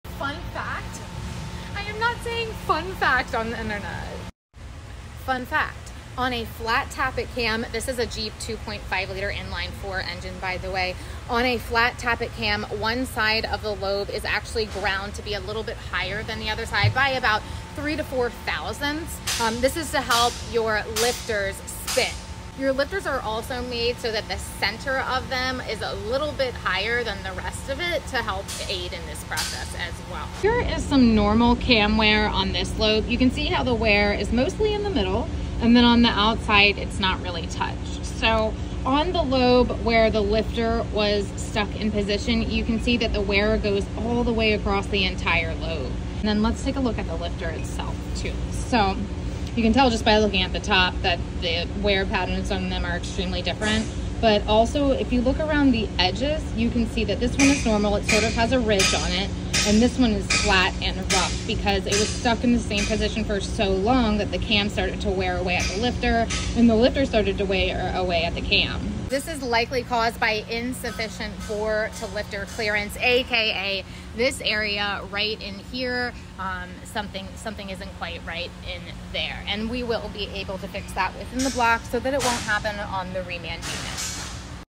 Flat Tappet cam and lifter wear
This Jeep 2.5 L Inline 4 cylinder had a stuck lifter - we see a lot of cam and lifter west when it comes to hemis and any MDS/DOD engine ￼but it happens to the non rollers too!